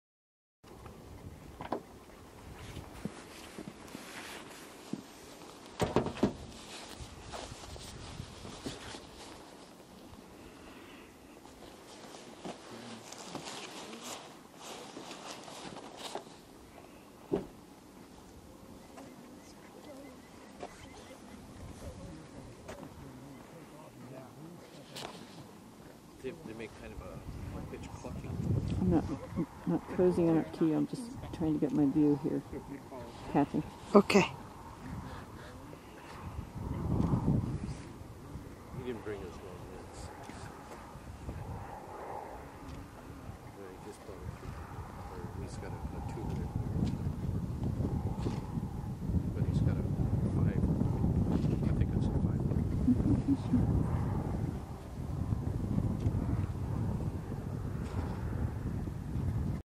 Sharp-tailed Grouse on lek near Sault Ste. Marie, Michigan
Taken on 02/05/2011 on a Washtenaw and Jackson Audubon field trip to the Sault Ste. Marie, Michigan area to see winter birds.